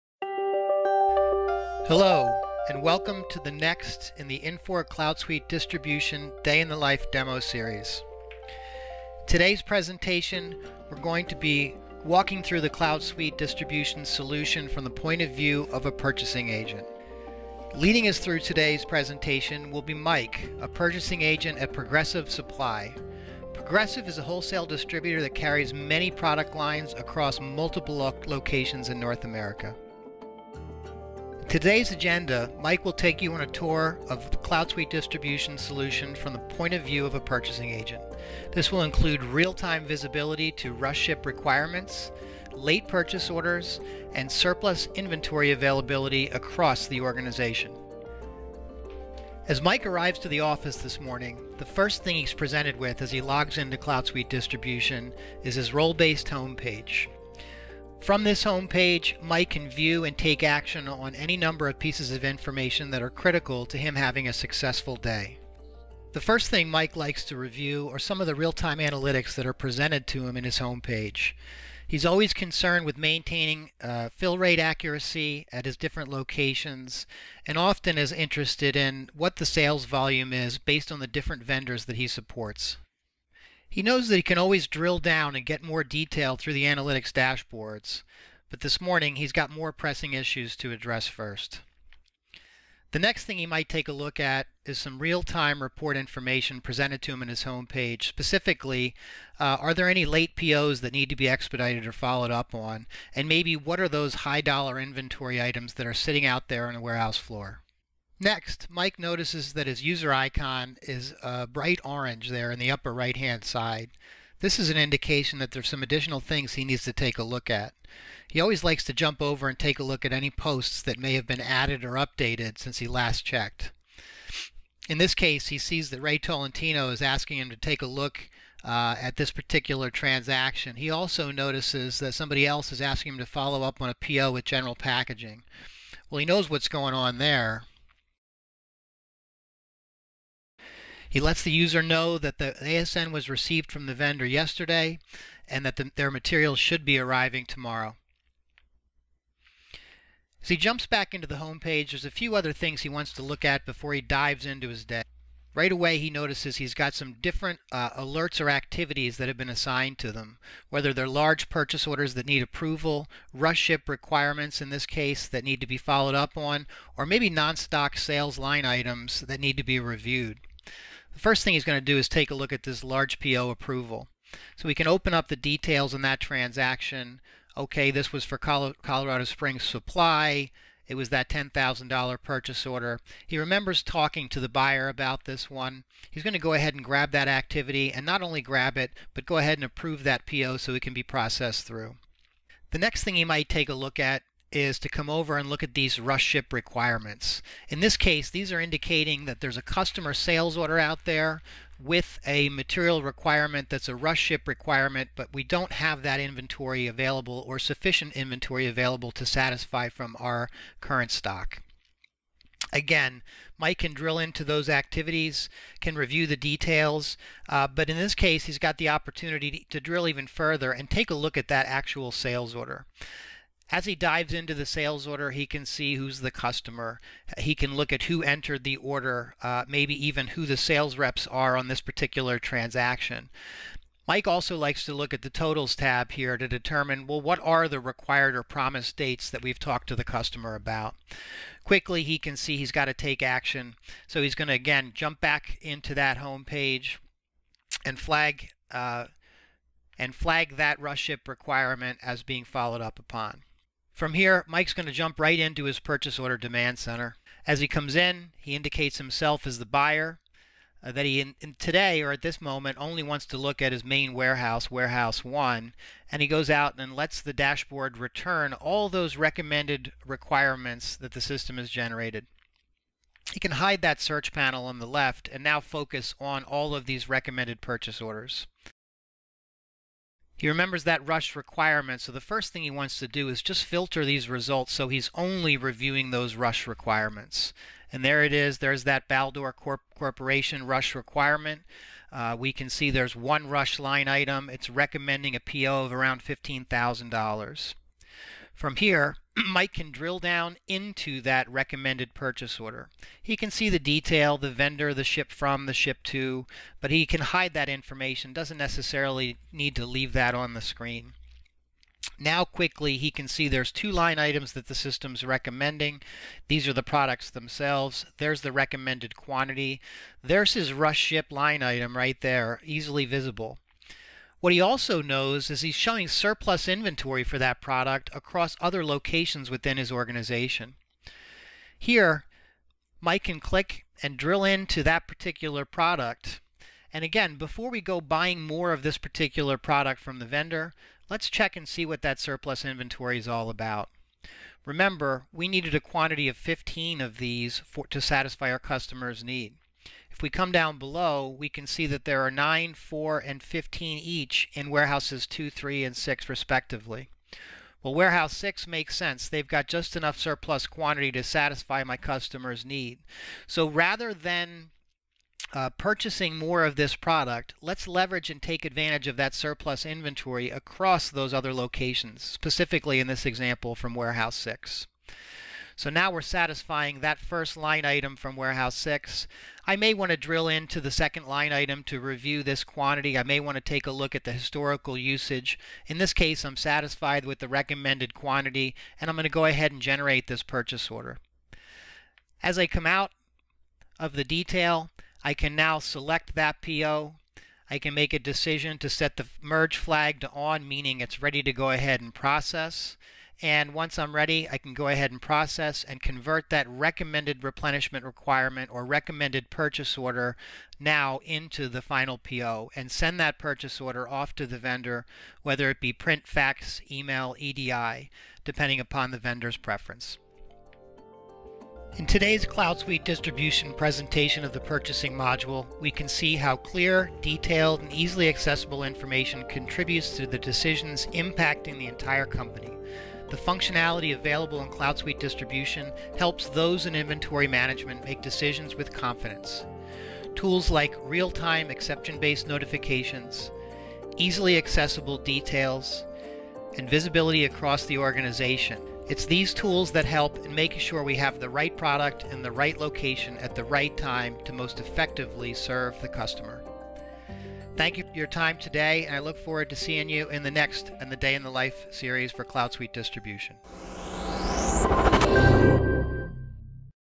Infor Tutorial: Day in the Life of a Purchasing Agent
In this video, we will take you through a day in the life of a purchasing agent as he leverages the tools available in CloudSuite Distribution.